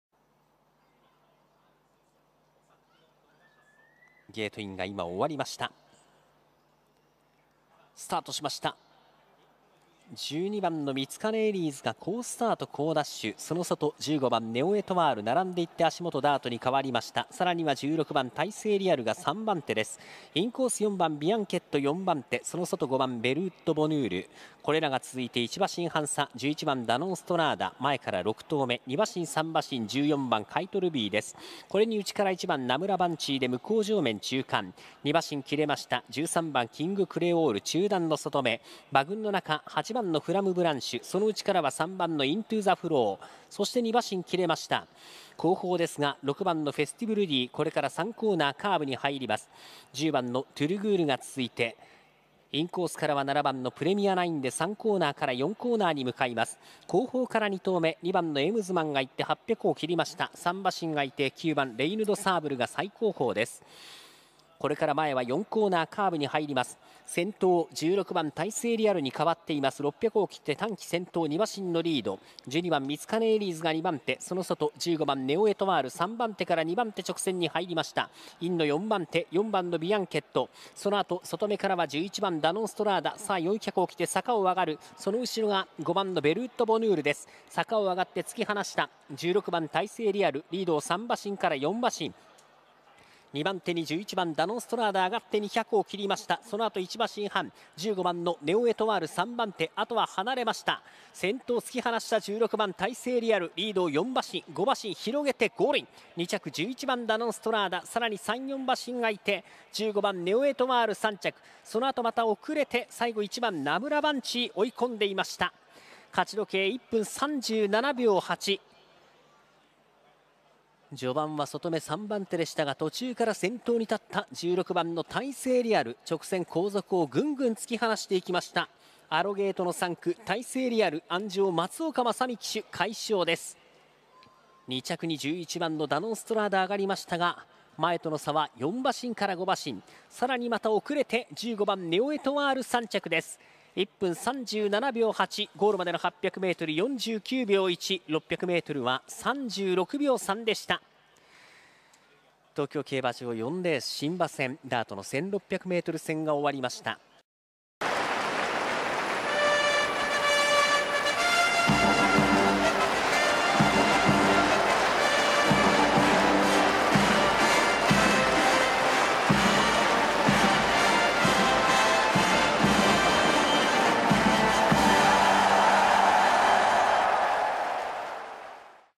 2歳新馬 ダ1600m (左) タイセイリアル 牡2 56.0 松岡正海 2023.11.19 出走馬：ダノンストラーダ、ネオエトワール、ナムラバンチー、フラムブランシュ、ミツカネエリーズ他
2歳新馬

11:25発走 / ダ1600m (左) / 天候:晴 / 馬場:稍
5回 東京 6日目 サラ系２歳 新馬       [指] 馬齢 16頭

タイセイリアル
生年月日 2021年2月20日
調教師 矢作芳人 (栗東)
馬主 田中成奉
生産者 下河辺牧場
産地 日高町
セリ取引価格 -
獲得賞金 720万円 (中央)
通算成績 1戦1勝 [1-0-0-0]
主な勝鞍 23'２歳新馬
近親馬